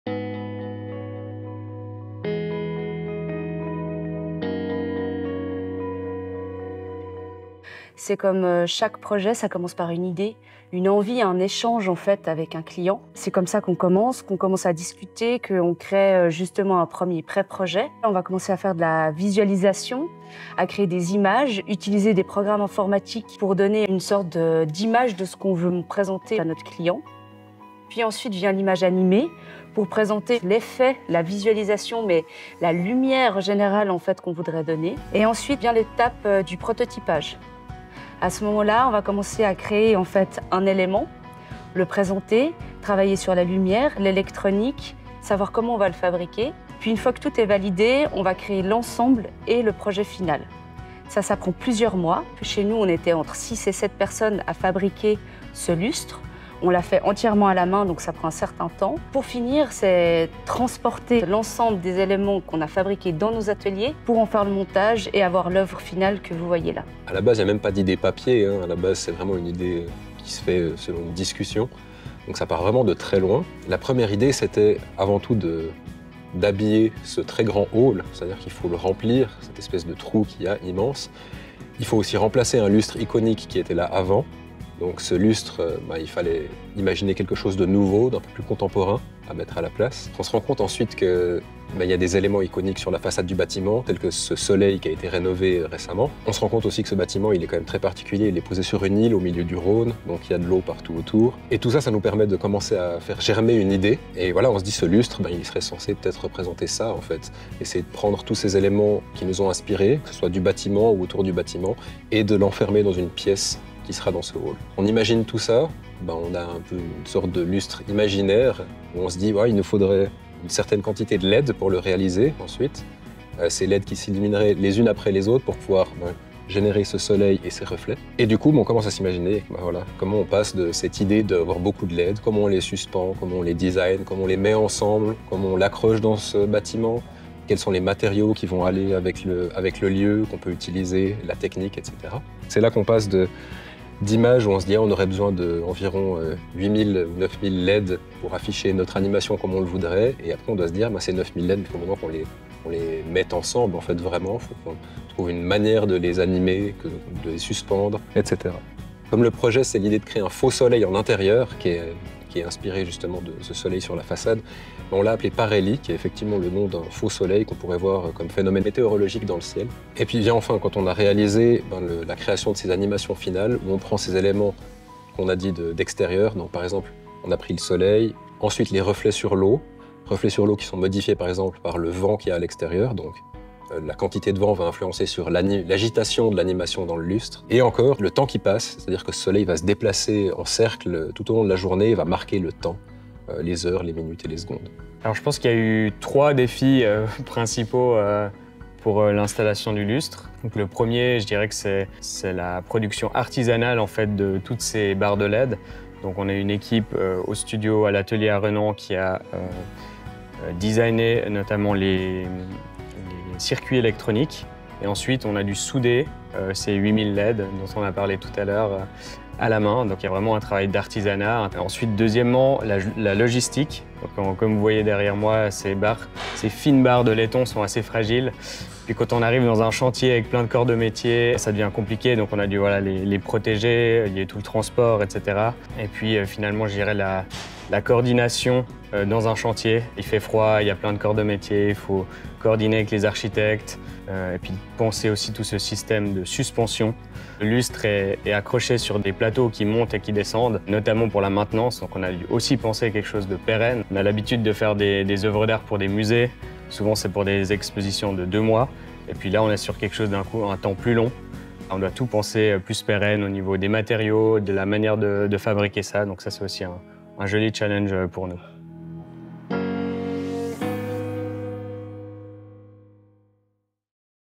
BCGE I Histoires d'une métamorphose : Le lustre "Parhélie"
Le trio de créateurs, contributeur de la rénovation du siège de la Banque, partage son témoignage sur la modernisation du monument historique situé en plein coeur de la Rade.

Production  @Fairymotion